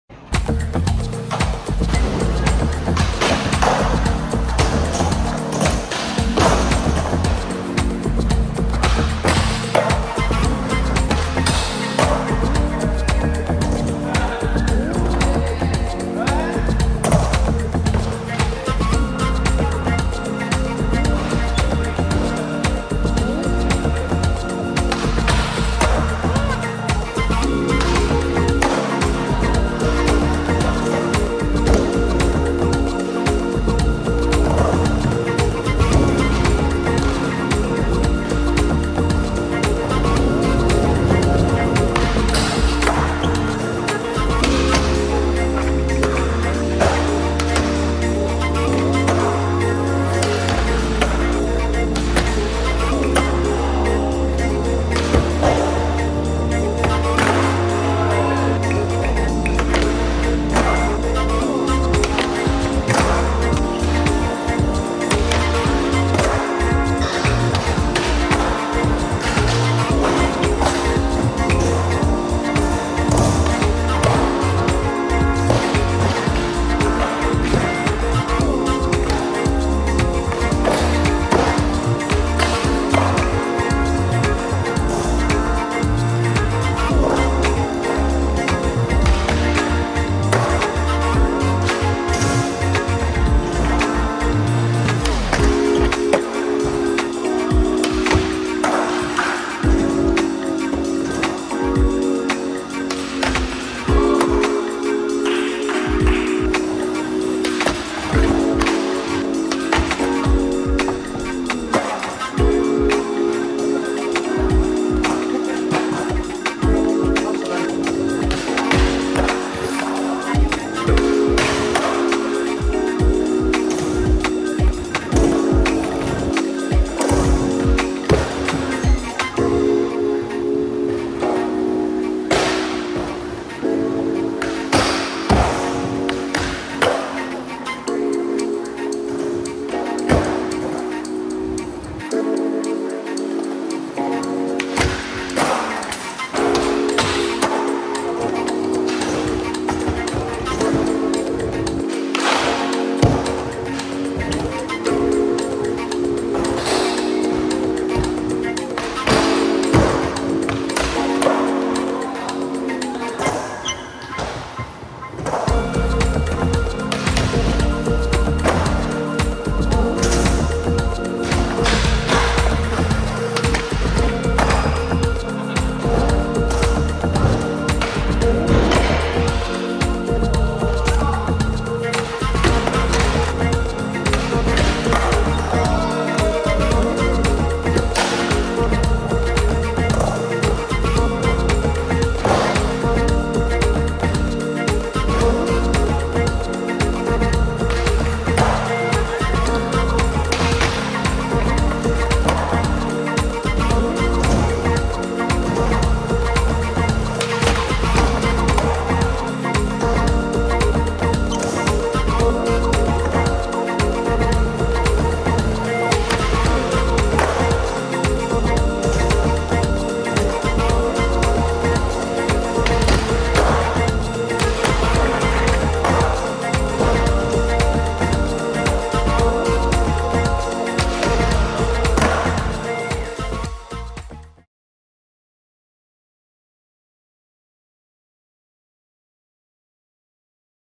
【山形 245パークに行ってきた 】Skate at best indoor skatepark in Yamagata!!
全国大会も開催される山形県のインドアパーク、245パークさんにお邪魔してセッションしてきました。質の高いセクションと鬼滑るロングカーブが超気持ちいい！
ローカルの皆さん、ありがとうございました。